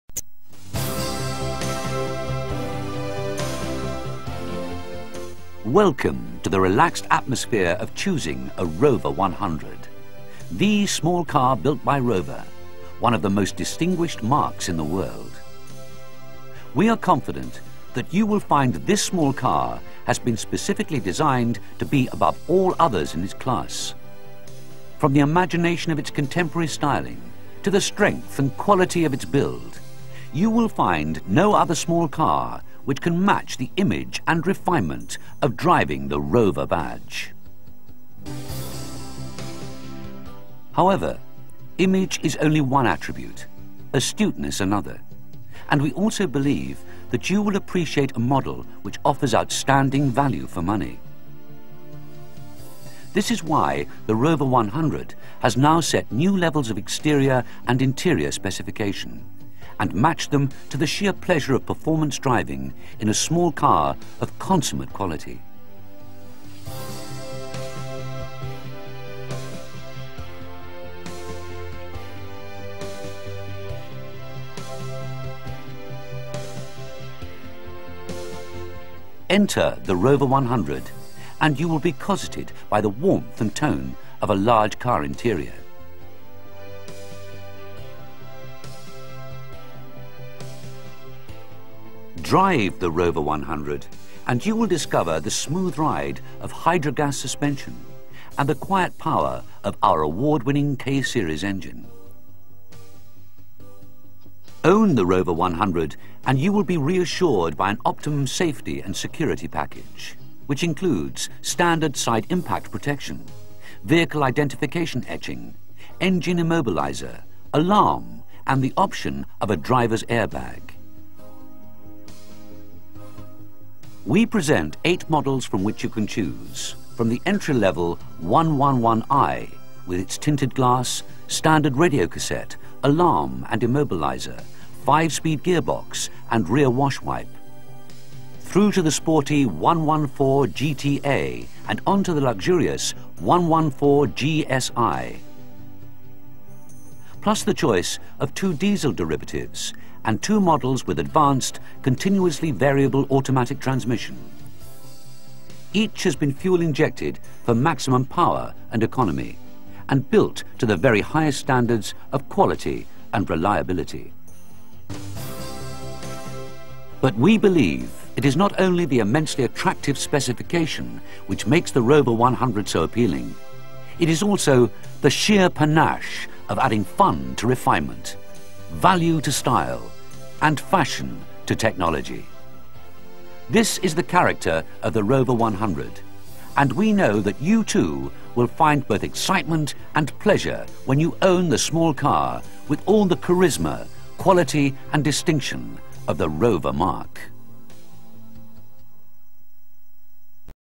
Rover Group - Rover 100 Series
A showroom introductory sales video from 1996 featuring the Rover 100.